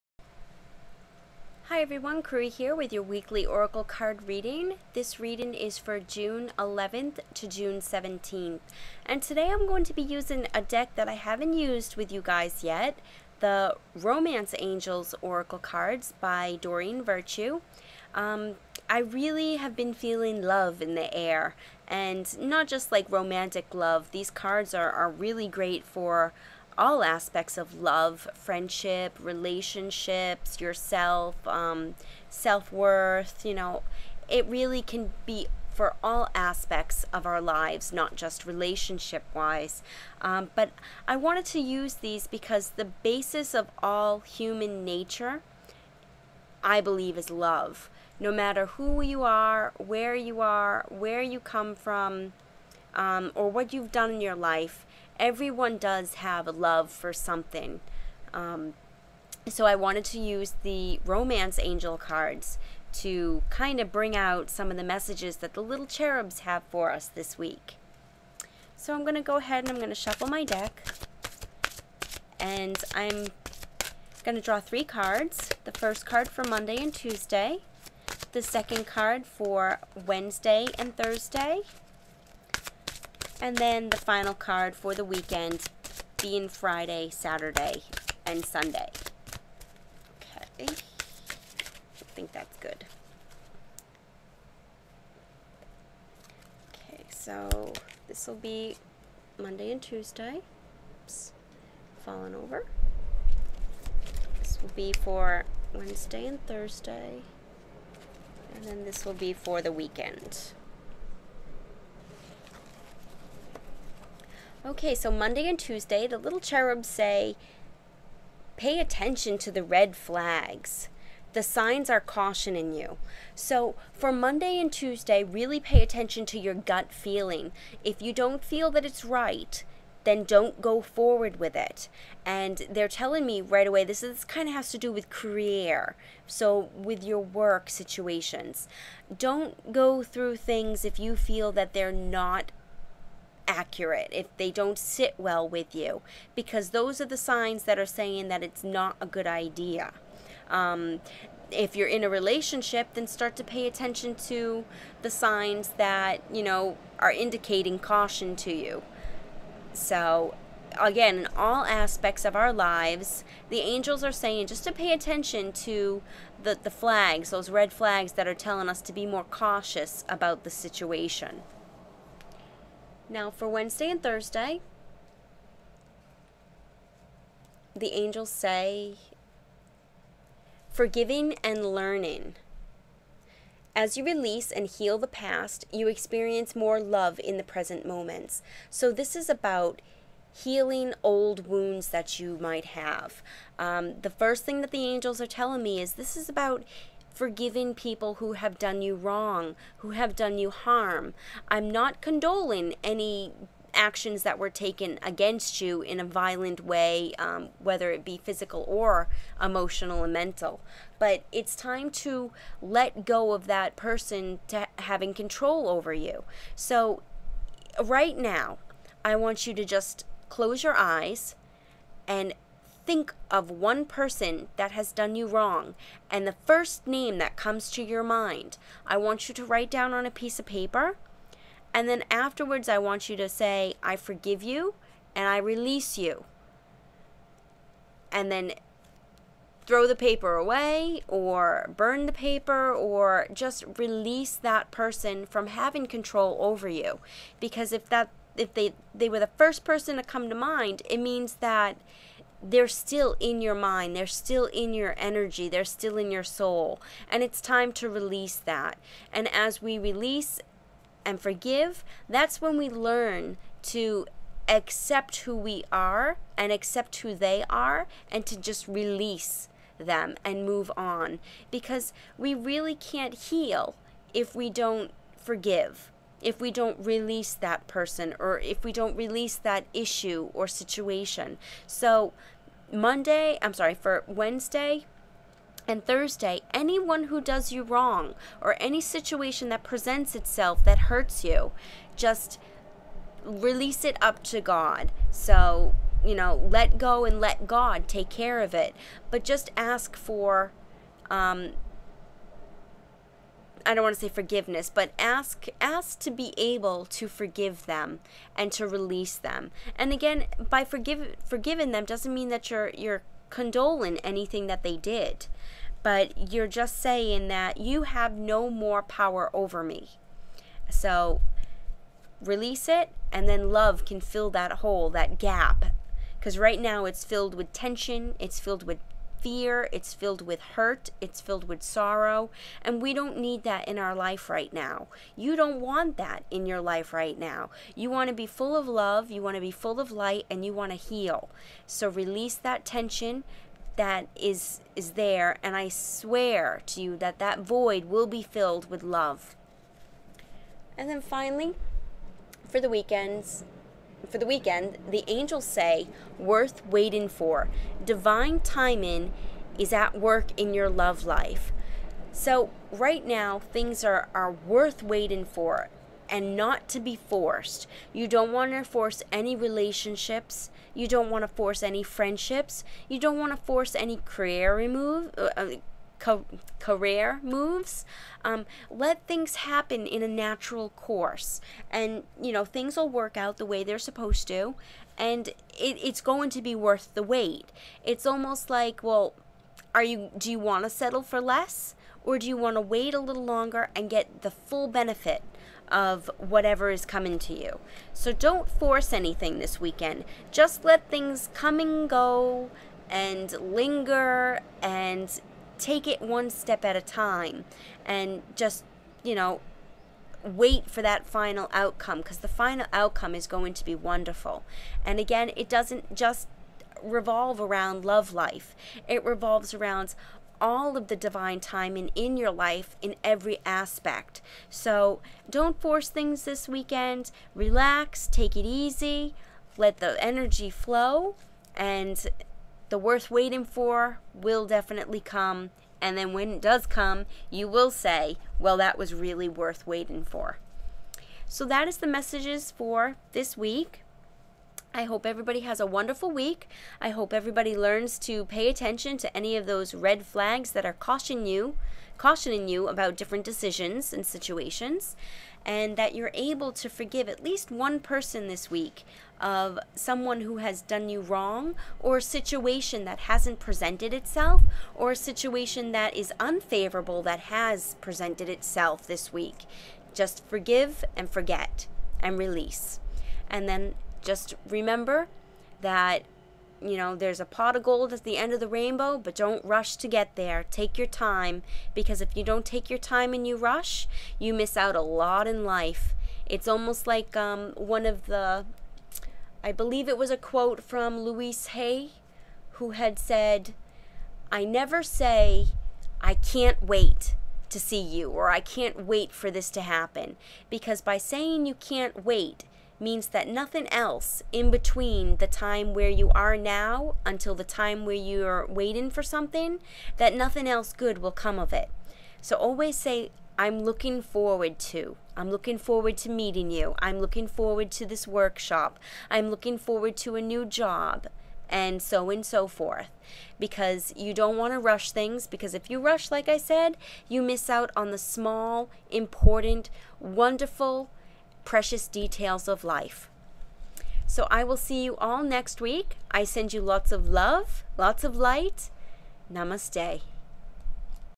Weekly Reading June. 11th- June 17th
Hi Everyone! Here is your weekly Oracle card reading for June 11th to June 17th.

Monday and Tuesday the angels want us to pay attention to the red flags. Make decisions using your gut feeling. If it doesn't feel right than don't proceed. The angels will send you clear signs of caution.
Wednesday and Thursday they tell us to forgive and learn to heal. By forgiving and releasing old negative experiences or people, we make room in our hearts to fill with love so that we can have more love in our life.
For Friday, Saturday, and Sunday, Don't rush things or try to make things happen. Our final outcome in all situations is all in Divine Order and timing. Relax, and be patient because the outcome is 'Worth Waiting For'.
I hope you all have a wonderful week. I send you all lots of LOVE and LIGHT!